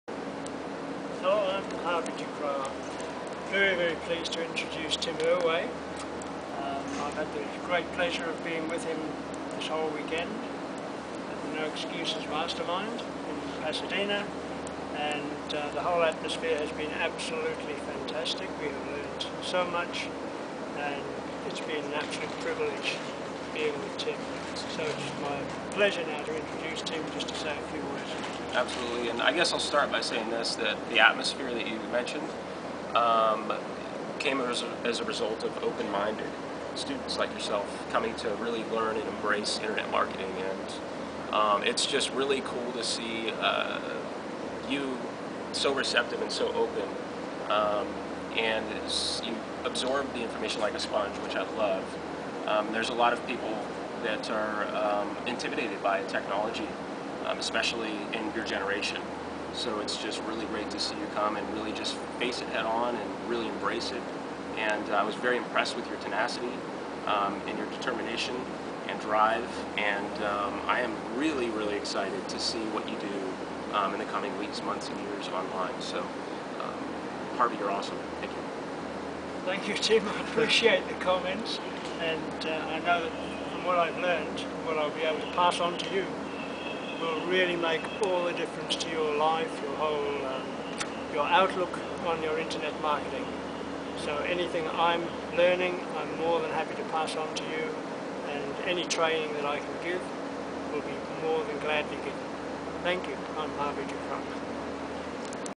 "NO EXCUSES 2" MARKETING MASTERMIND - VIDEO 2
DYNAMIC ATTRACTION - VIDEO INTERVIEW WITH TIM ERWAY
A short video taken at the "No Excuses 2" Marketing Mastermind held in Pasadena in July 2011.